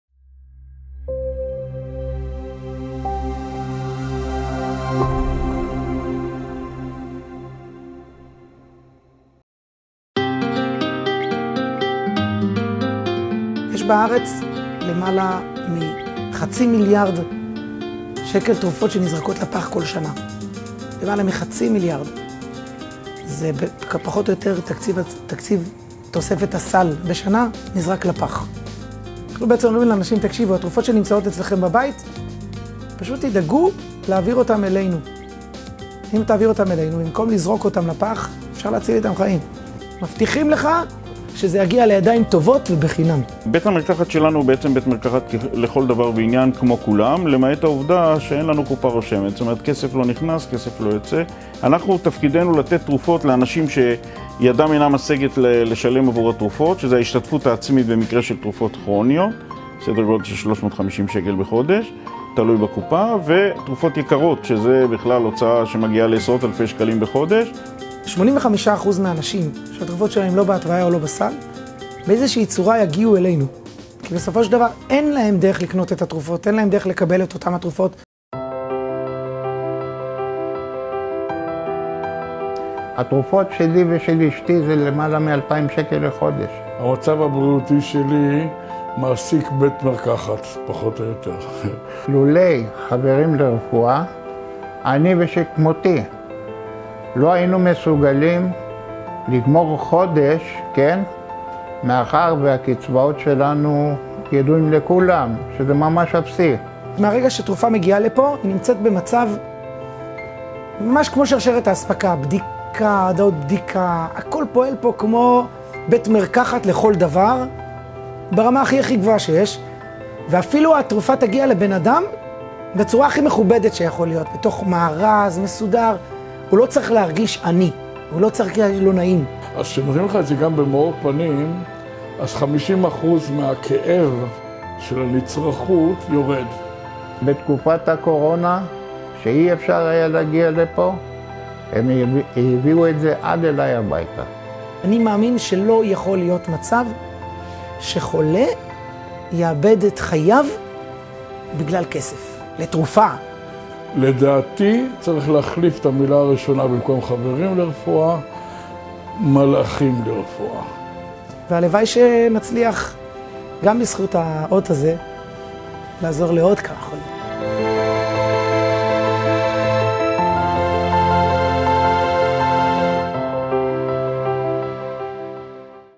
חברים לרפואה: אות שר הבריאות 2020
עמותת חברים לרפואה היא הזוכה ב'אות שר הבריאות למקדמי זכויות במערכת הבריאות' בקטגוריית ארגונים חברתיים גדולים בשנת 2020.

העמותה עוסקת באיסוף תרופות ללא שימוש ובהעברתן למי שידם אינה משגת.
תרופות שאילולא האיסוף היו נזרקות לזבל. העמותה מפעילה בית מרקחת לכל דבר העומד בכל אמות המקצועיות והאיכות, מלבד העובדה שהתרופות המונפקות בו אינן כרוכות בתשלום.